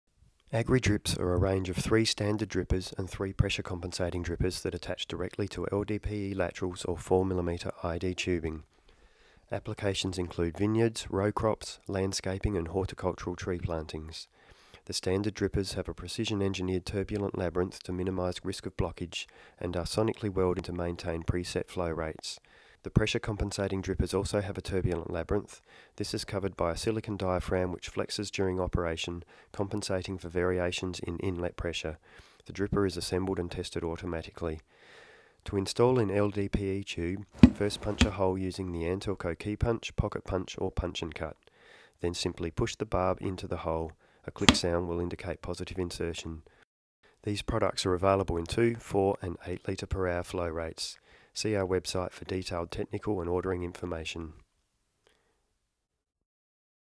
Agri Drip Emitters Guide - Drip Irrigation
View the short guide for the Antelco Agri Drip Emitters for Drip Irrigation. The video includes information on key features, and how to install correctly.

Agri drips are a range of three standard drippers, and three pressure compensating drippers that attach directly to LDPE laterals or 4mm id tubing. Applications include vineyards, row crops, landscaping, and horticultural tree plantings. The standard drippers have a precision engineered turbulent labyrinth to minimise risk of blockage, and are sonically welded to maintain preset flow rates. The pressure compensating drippers also have a turbulent labyrinth. This is covered by a silicon diaphragm which flexes during operation, compensating for variations in inlet pressure. The dripper is assembled and tested automatically.

To install in LDPE tube, first punch a hole using the Antelco key punch, pocket punch, or punch n cut. Then simply push the barb into the hole; a click sound will indicate positive insertion. These products are available in 2, 4, and 8 litre per hour flow rates.

See our website for detailed technical and ordering information.